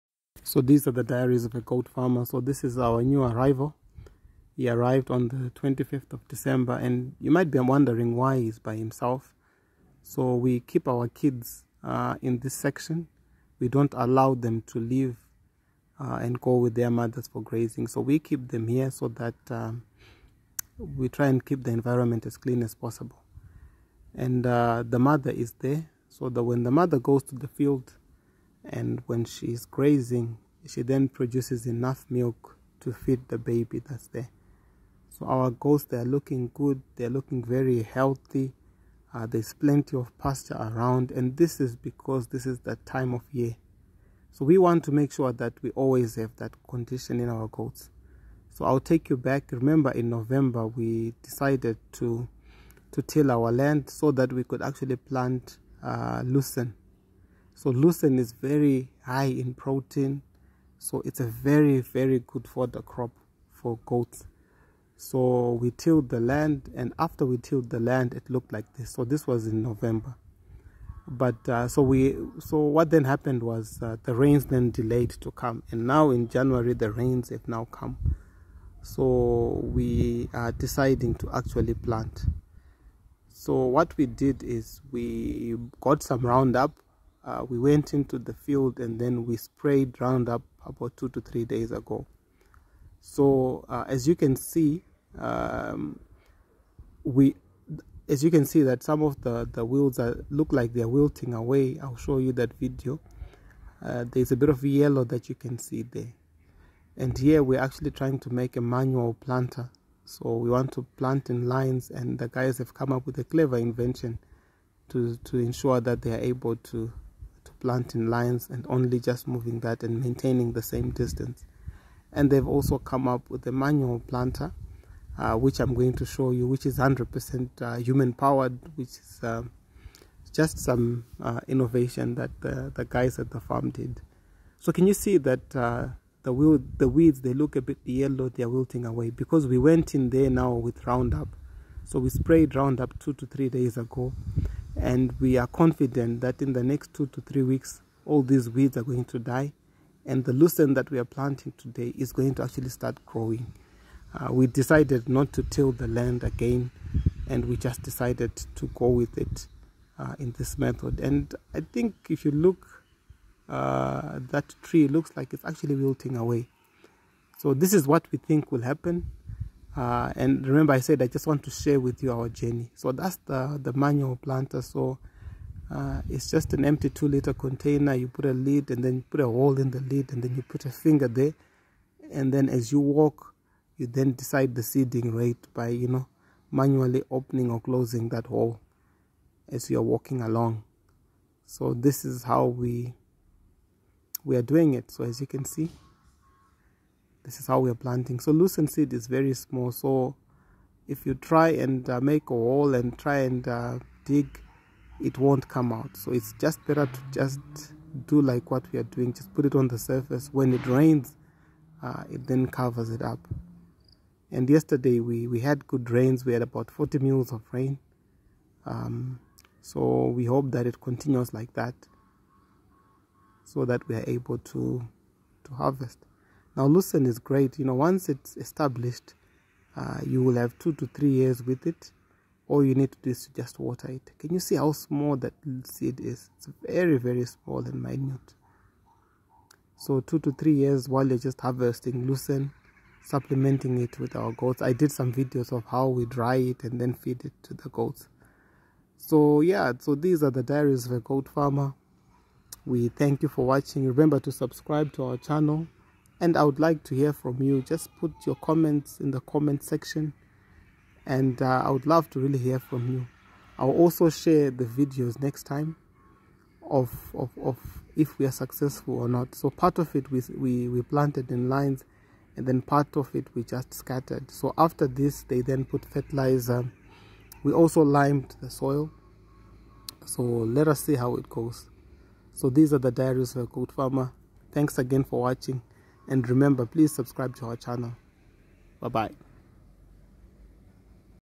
Rain is Good for Goat 🐐 Nutrition! - Make Lucern while it's still raining!
We describe how we are utilising the rainy season for grazing and also for planting crops that will produce food for our goats in the dry season!